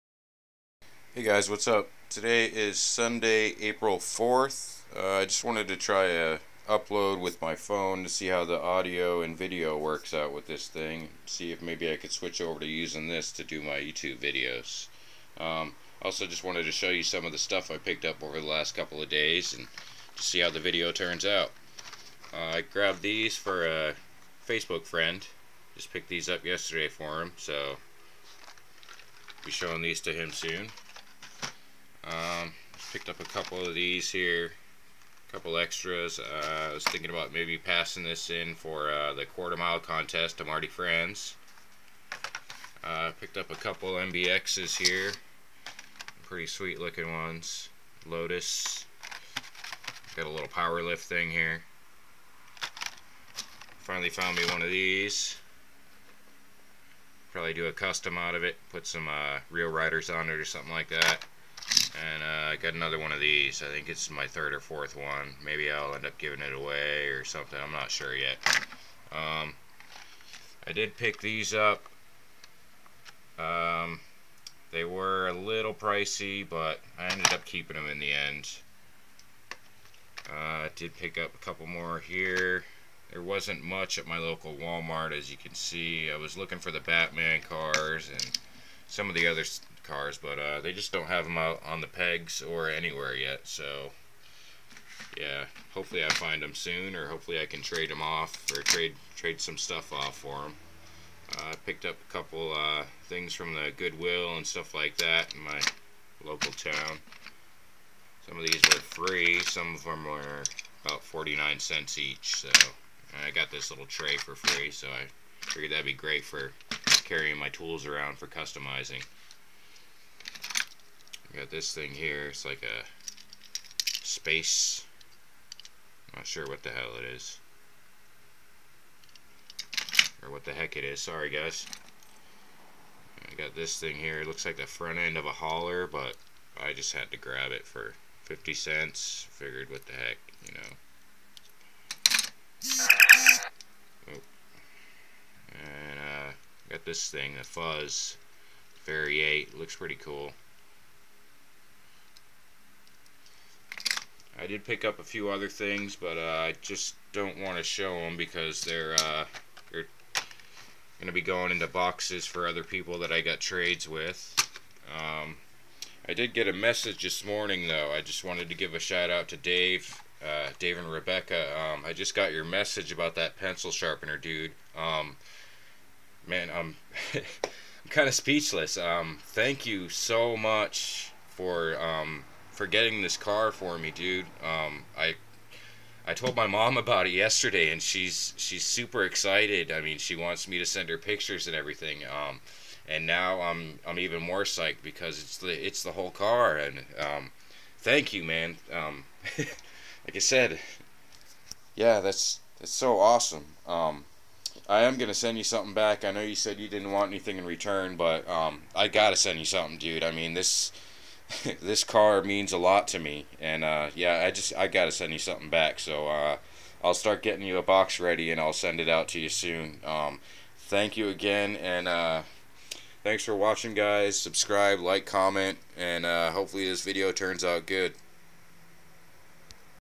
Mini haul/Shout out (video test)
Just a quick video test from my phone. Also a quick thank you shout out. The car is NOT in this video, it's on it's way to me now. Unboxing of it will be in a few days. Thank you Dave Rebecca :)